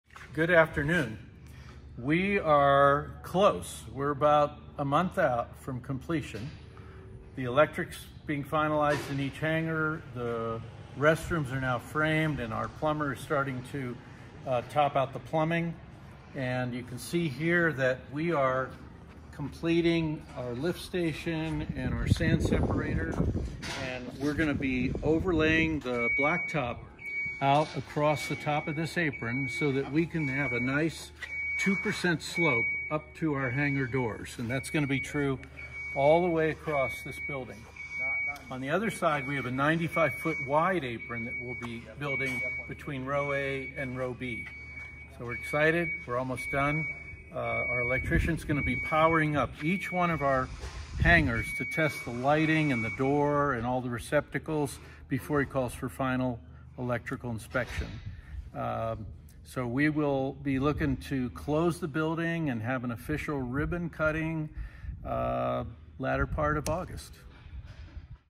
July 2024 Update • Double Eagle Aircraft Hangars
Kenny Hinkes shares the July 2024 update on the first row of General Aviation hangars nearing completion at Albuquerque's Double Eagle II airport. The construction project has progressed on schedule beginning in the Fall of 2023 and aircraft owners are eagerly anticipating the finish of construction in the coming months.